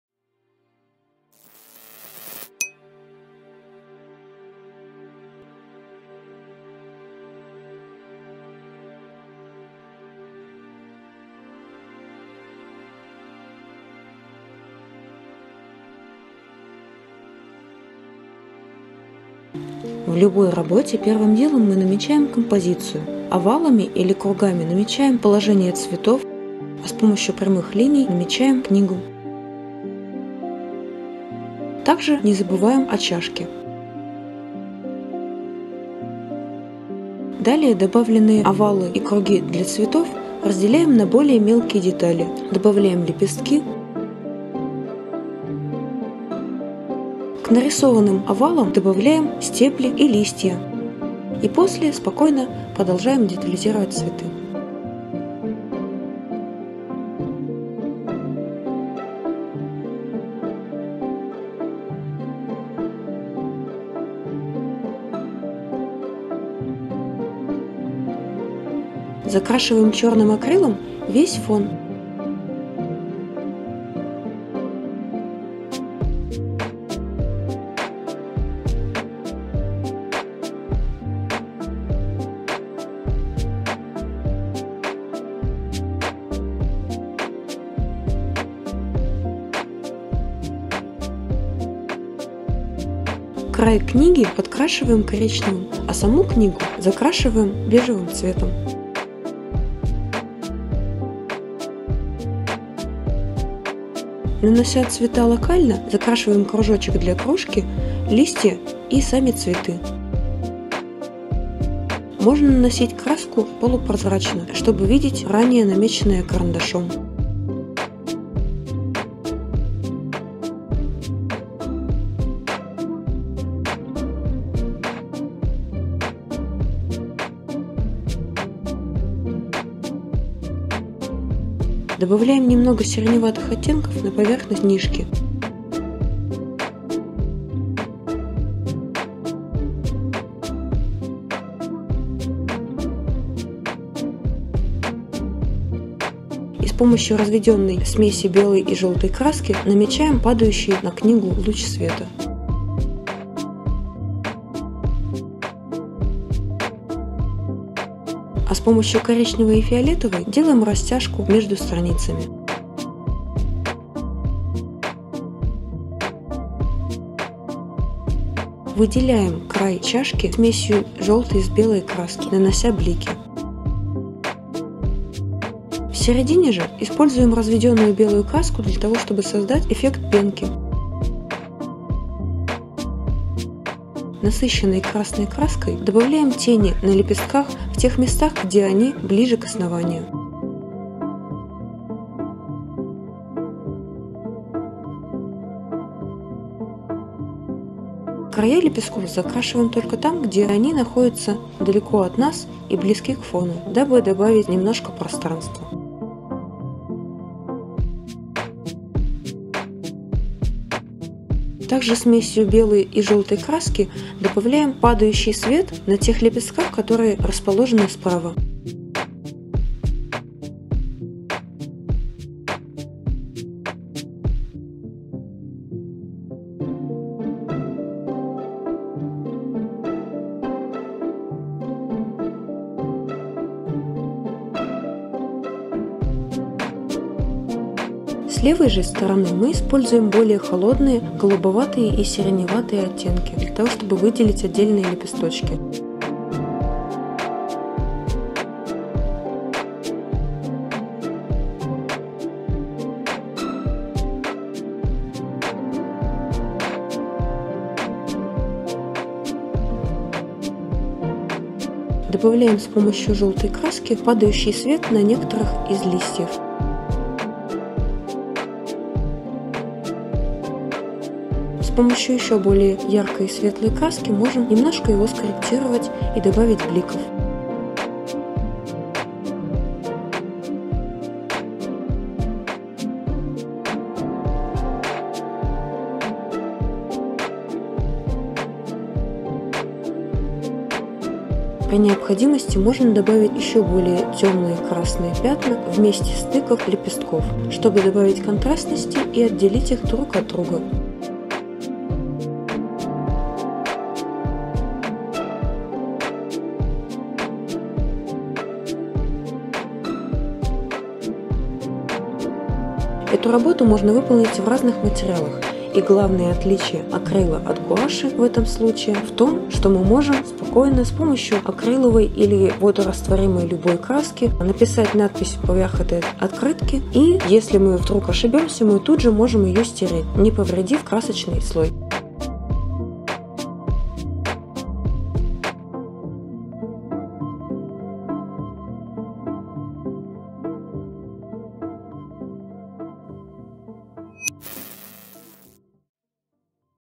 Как нарисовать открытку акрилом \ гуашью. Рисуем цветы, мастер-класс.
Поэтапное рисование открытки, подходящее для работы как акрилом, так и гуашью. Видеоурок с устными комментариями.
Буду благодарна, если вы напишите в комментариях о своем опыте рисования, что бы вы хотели улучшить в моих видеоуроках, понравилось ли вам и о том, что бы вы хотели нарисовать еще.
Спасибо за ваше внимание!